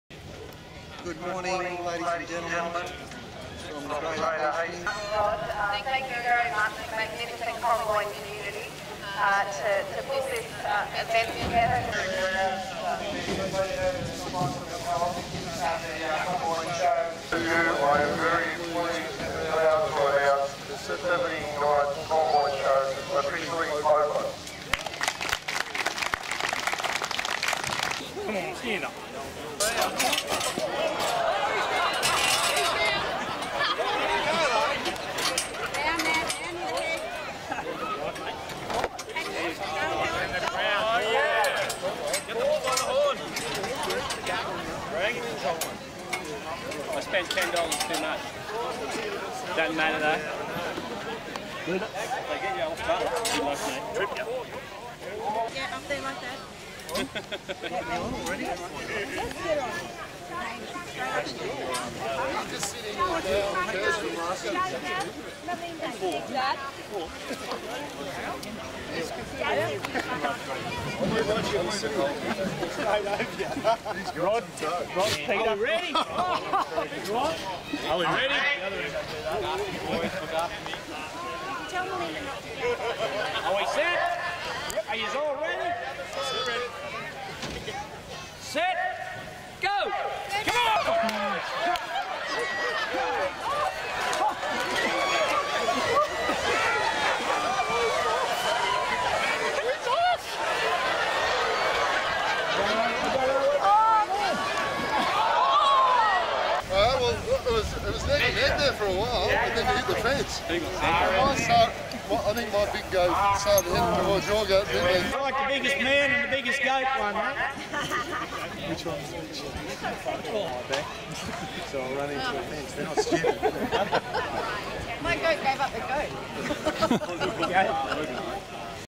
Comboyne Poly Goat Race 2016
This is a pre release of the 2016 Comboyne Poly Race, if anyone else has footage of this race and are willing to share it to improve the different view angles in the final version it would be much appreciated. Your Contribution be added to the credits.